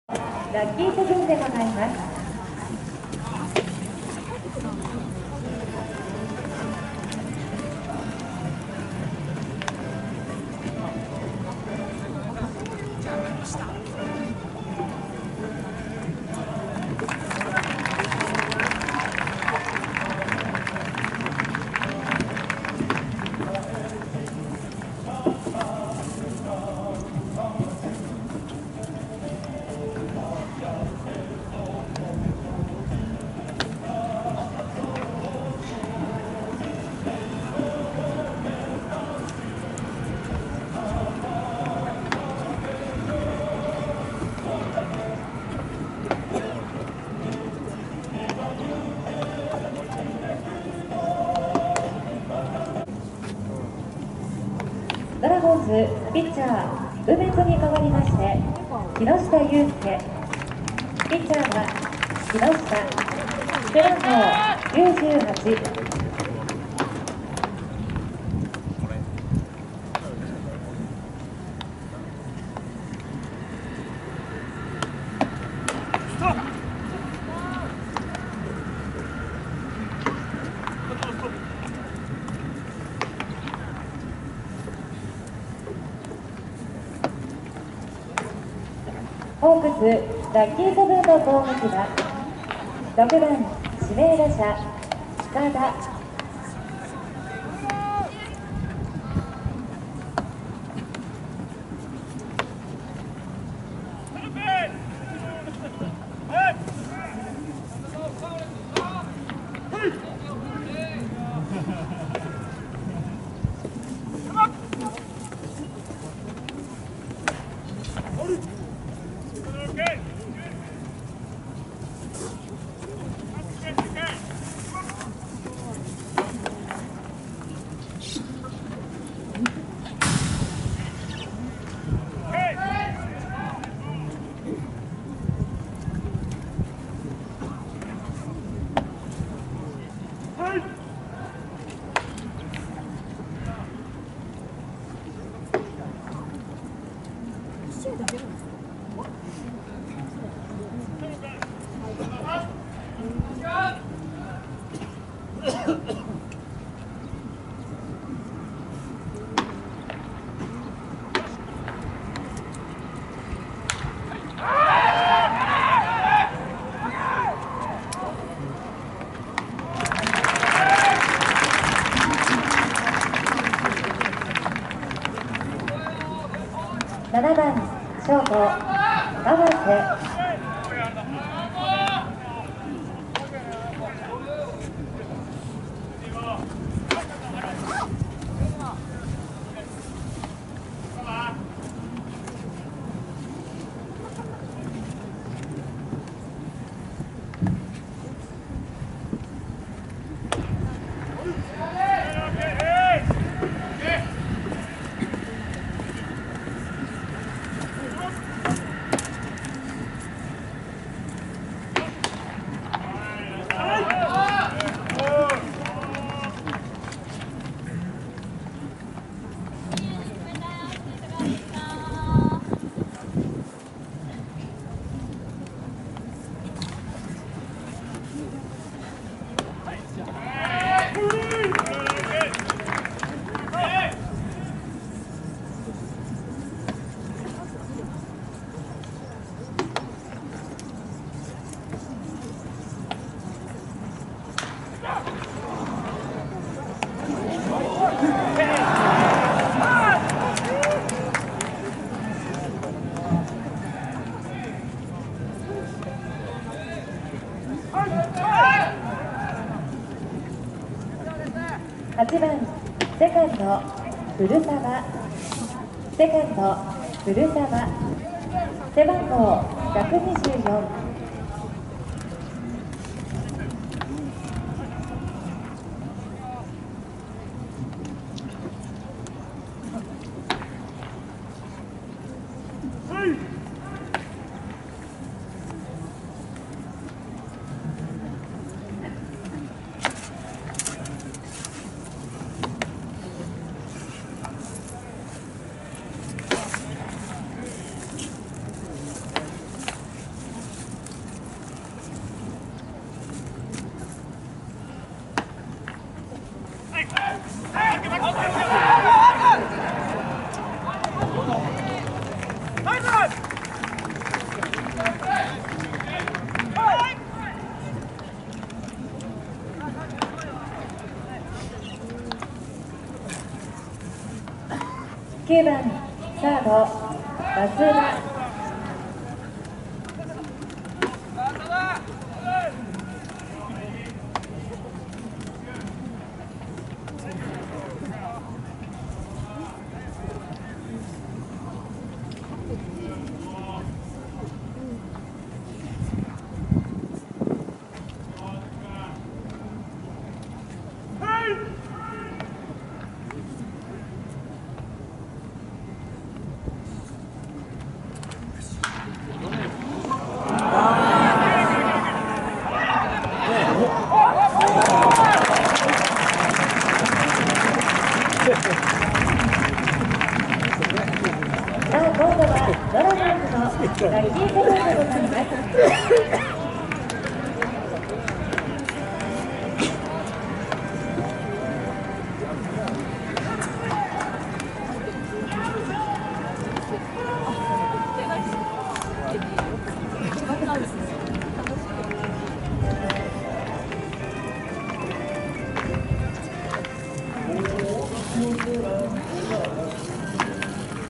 2019.5.22【中日ドラゴンズ２軍戦】木下雄介 1回・被安打1・無失点
ナゴヤ球場 中日ドラゴンズvs福岡ソフトバンクホークス(ウエスタン・リーグ公式戦)
7回表から登板